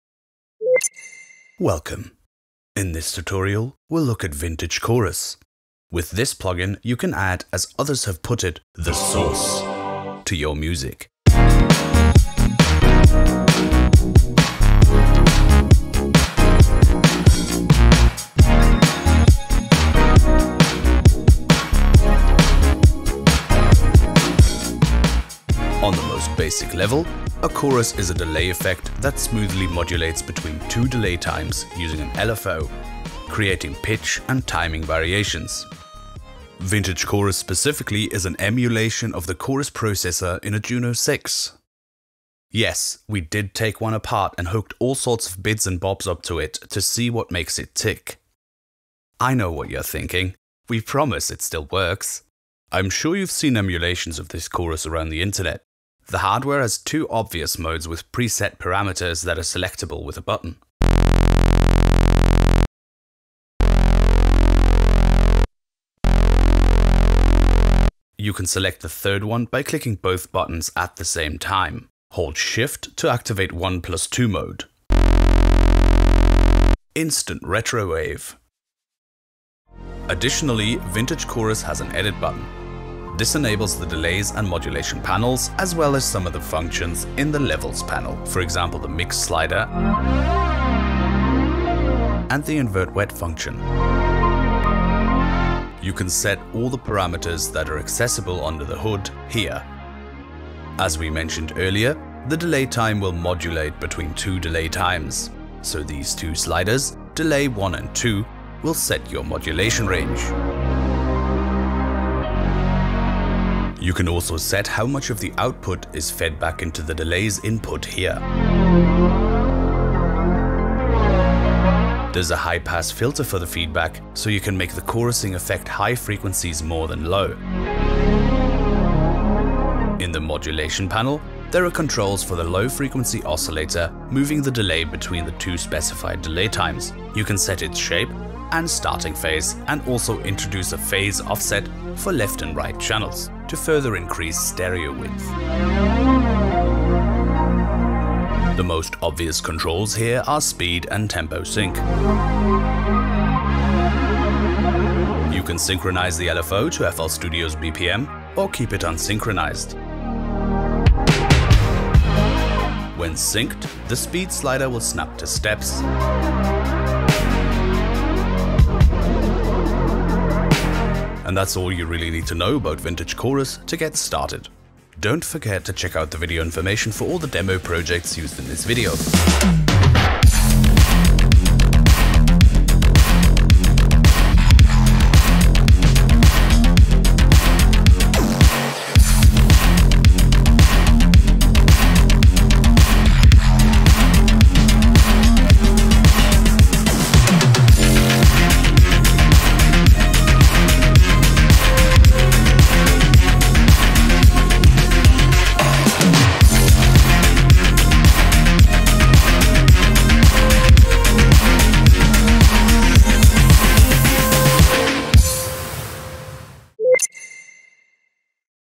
FL STUDIO | Introducing Vintage Chorus
Video Index:

00:00 Introduction

00:11 Basic Sound Demo
00:24 Concept: Chorus
00:36 Inspiration for Vintage Chorus
00:51 Vintage Chorus: Basic Functions
01:18 Vintage Chorus: Edit Mode
02: 50 Outro

Image-Line Software